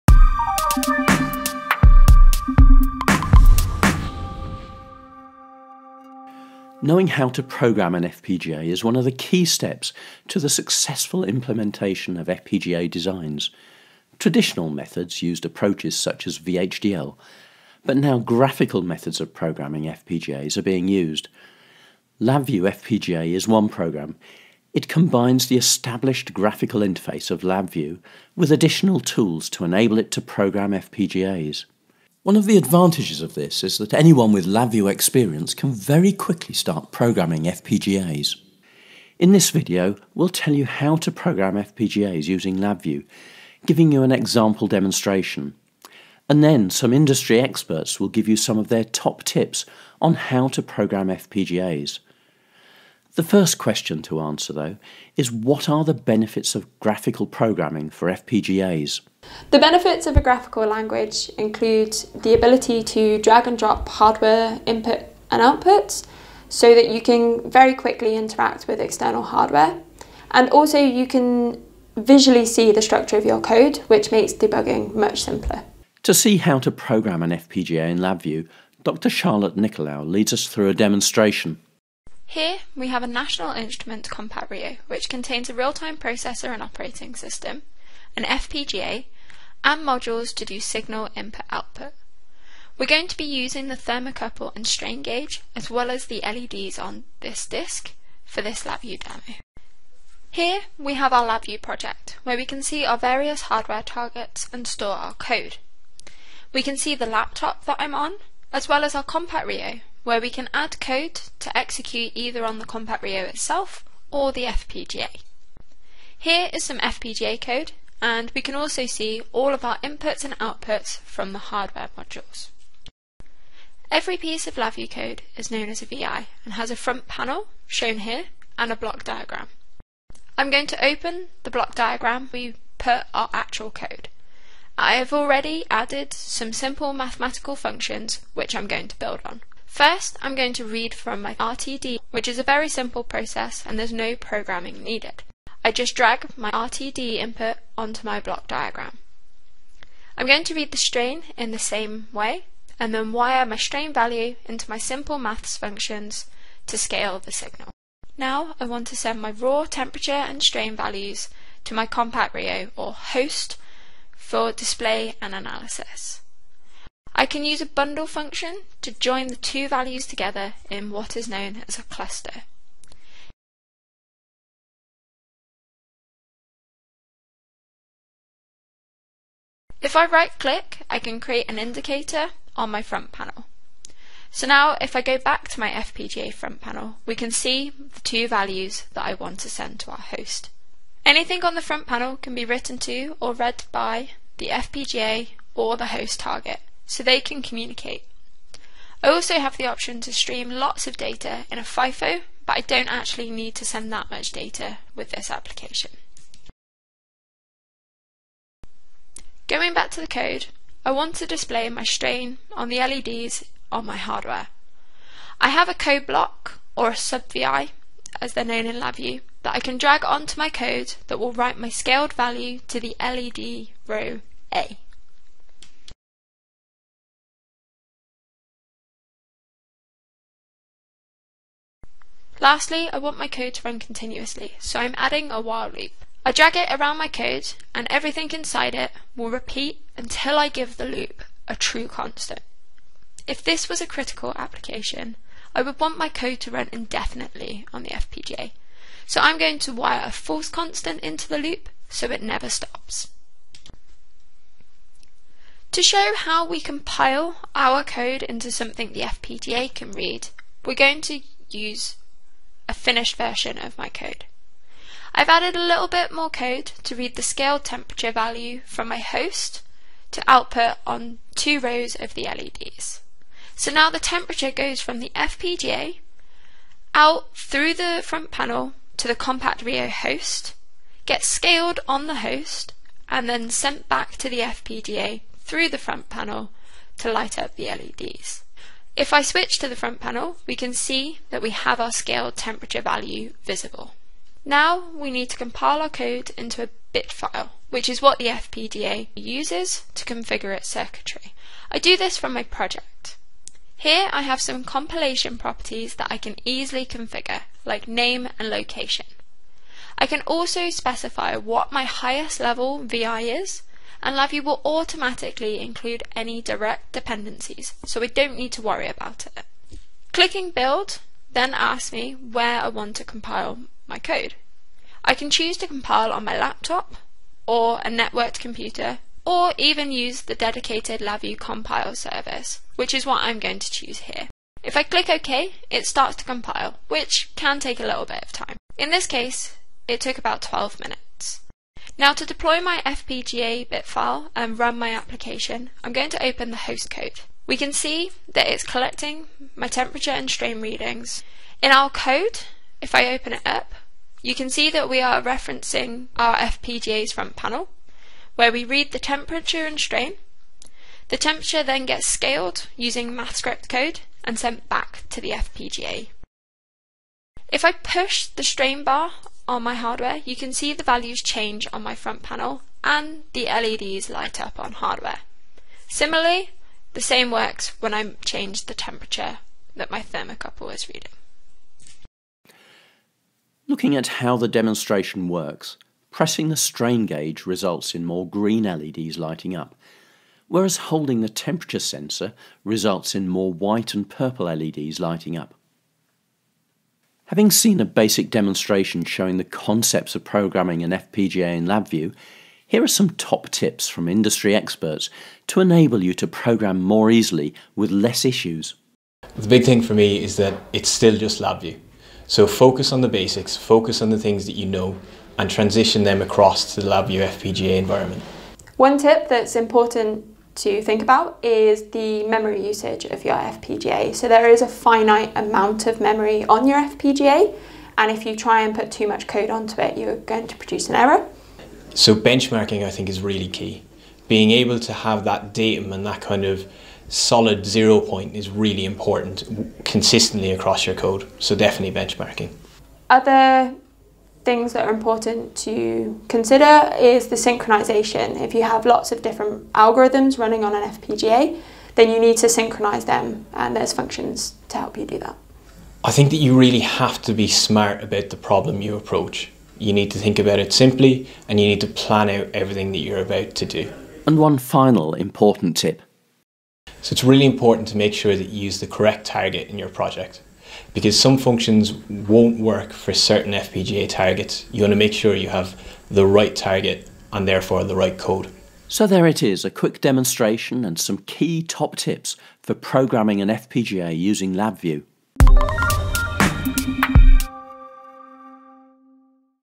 How to Program an FPGA with LabVIEW FPGA
Knowing how to programme an FPGA is one of the key steps to the successful implementation of FPGA designs. Traditional methods used approaches such as VHDL but now graphical methods of programming FPGAs are being used. LabVIEW FPGA is one programme. It combines the established graphical interface of LabVIEW with additional tools to enable it to programme FPGAs.

The video tells you how to program FPGAs using LabVIEW FPGA giving you an example demonstration. And then some industry experts give some of their top tips on how to programme FPGAs with LabVIEW FPGA.

FPGAs are used in many areas of the electronics industry. They are used where flexibility, and complex logic circuitry is required. FPGAs are also able to provide complex and fast processing and as such they are ideal for many applications from vision processing, and general signal processing to security, and in fact anywhere that complex logic circuitry is needed.

The key to a successful project is knowing how to program an FPGA properly. Although traditional methods of FPGA programming like VHDL have been used for many years and work well, graphical approaches are being increasingly investigated for FPGA programming. LabVIEW FPGA provides an ideal platform for many instances and it is based upon a tried and tested platform.

LabVIEW FPGA  utilises the basic LabVIEW platform. The addition of a few extra tools enables it to program FPGAs.

More information on LabVIEW FPGA and how to program an FPGA can be found